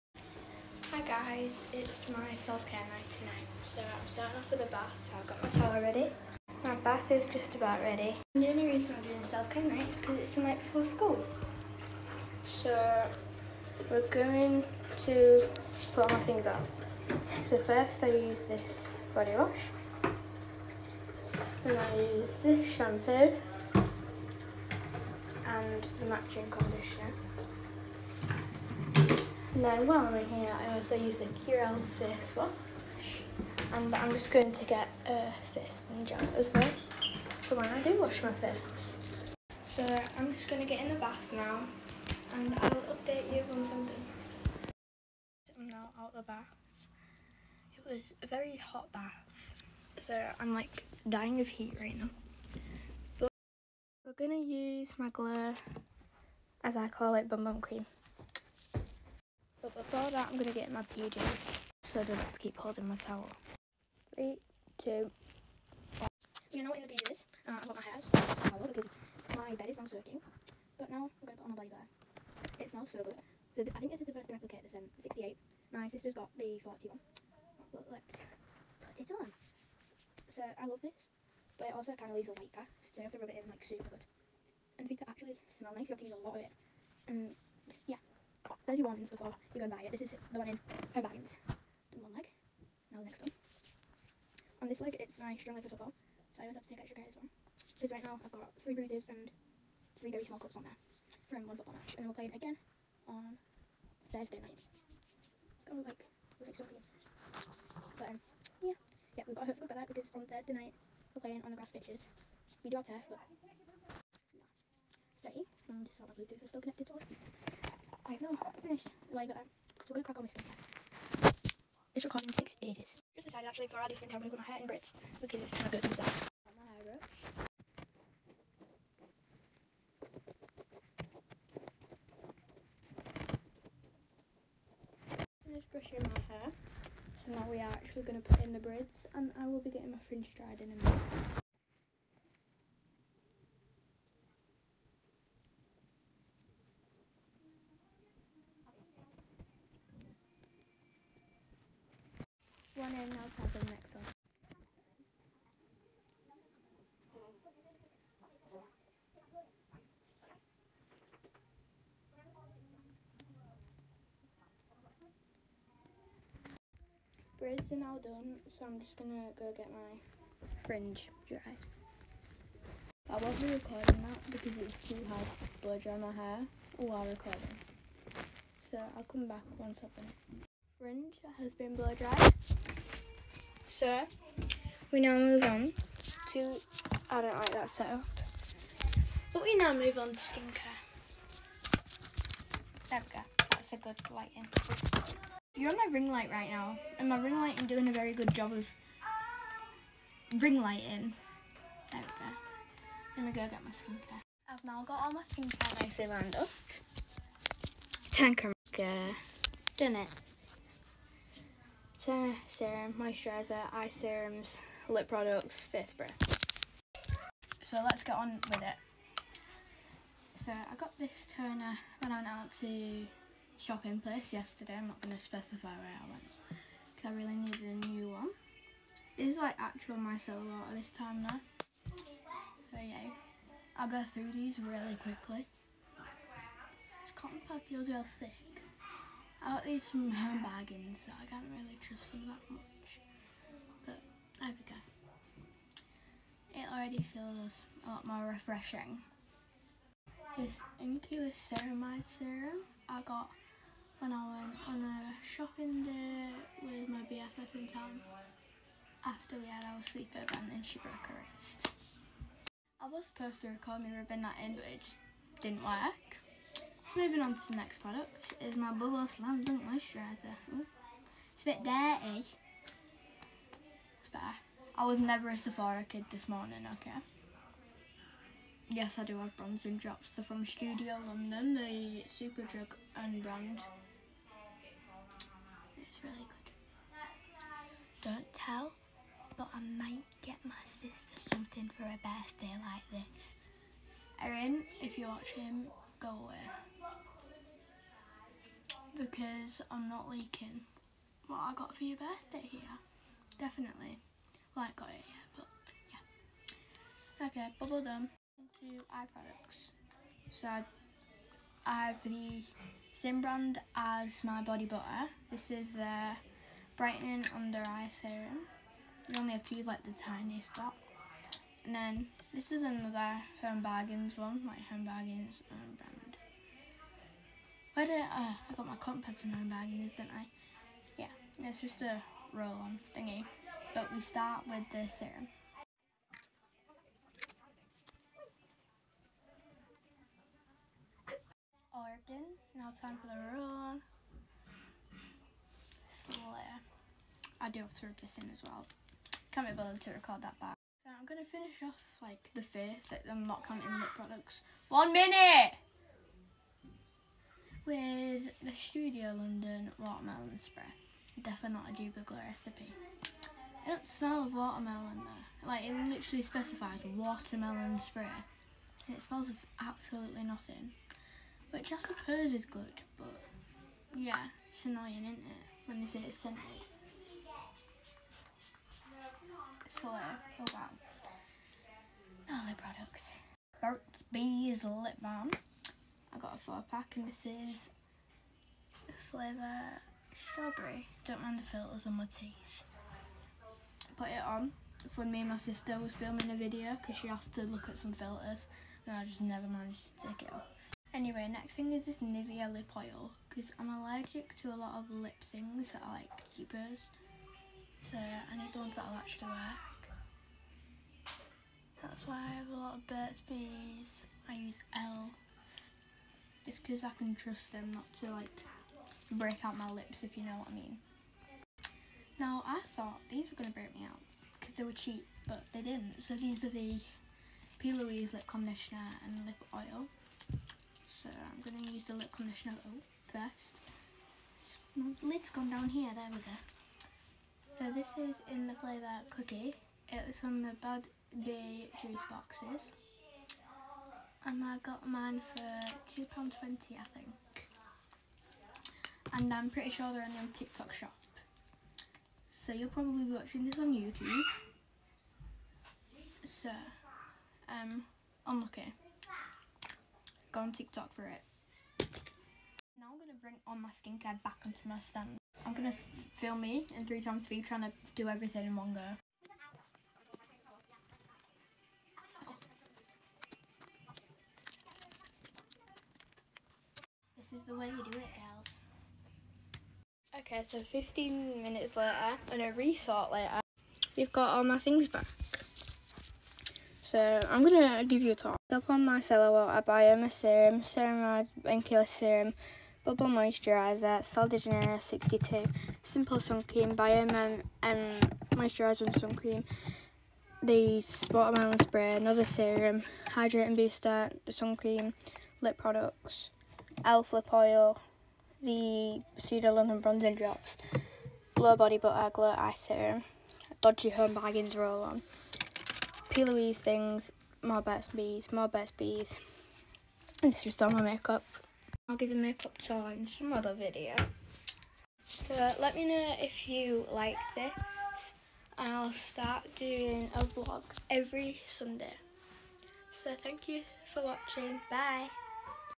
Self care night#viral
This video is a self care night before school for me,including my bath,hair and skincare routine